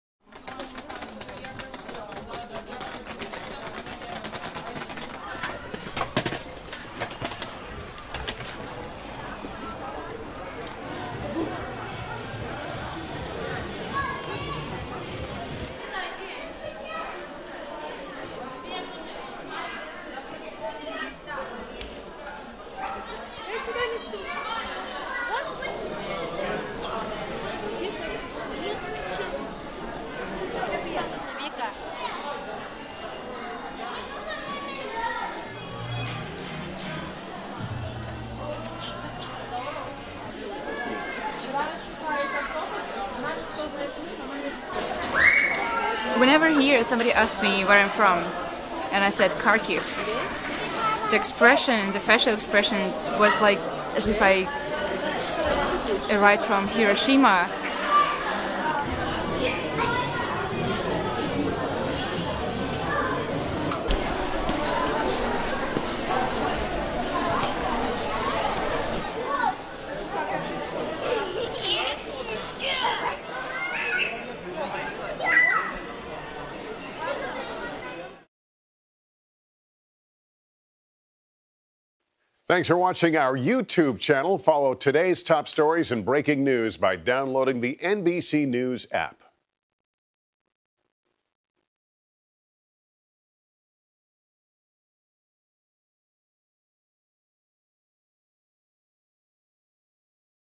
Romanian Hotel Turns Ballroom Into Ukraine Refugee Shelter
A hotel in Suceava, Romania, has turned its lavish ballroom into a makeshift shelter for more than 2,000 refugees who have fled Ukraine since the Russian began its invasion. According to the U.N., more than 1.7 million people have fled Ukraine in the last 10 days, making it the swiftest refugee exodus so far this century.